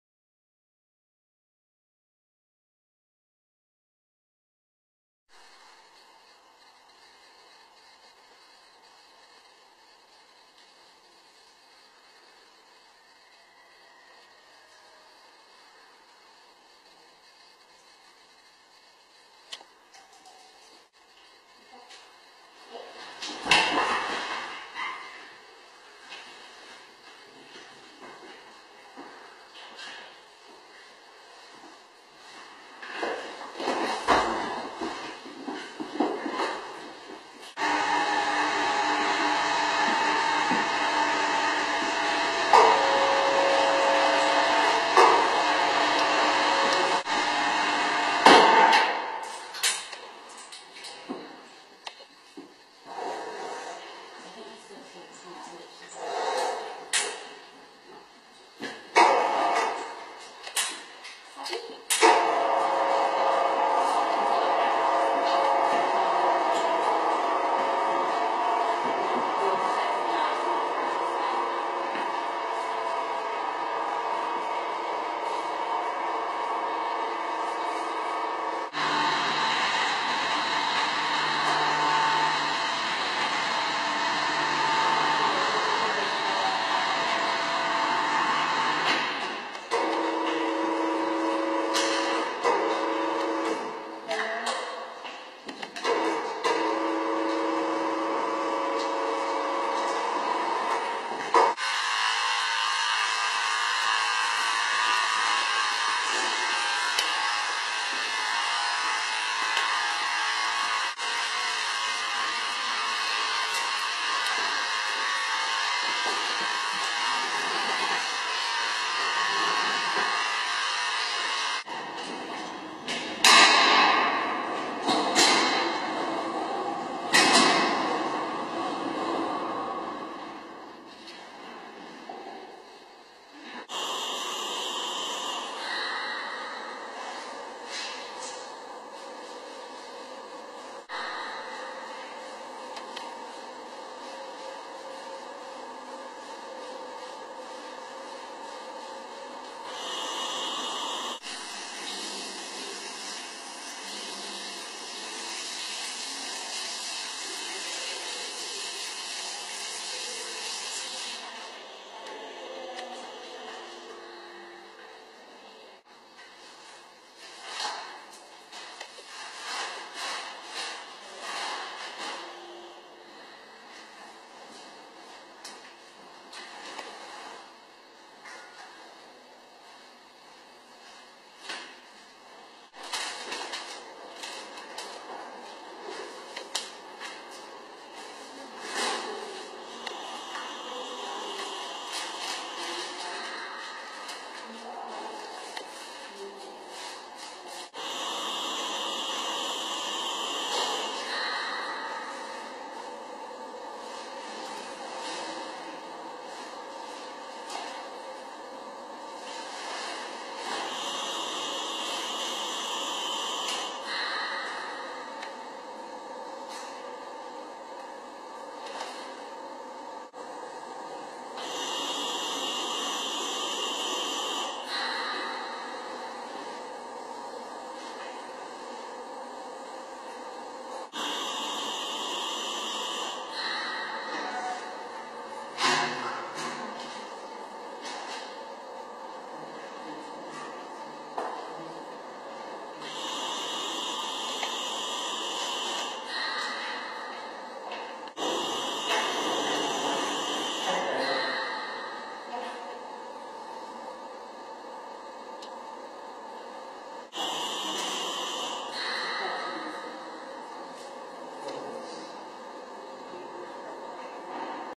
شاهد كيف تمت عملية جراحية لحصان
طاقم طبي وتخدير وغرفة طبية متطورة
عملية جراحية لحصان